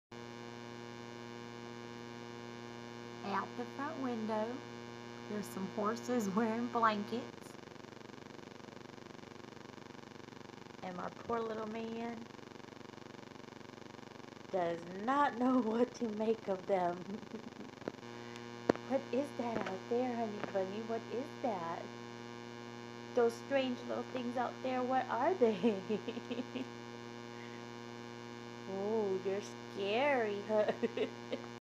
What's that outside?
My little Boston seeing horses with blankets for first time.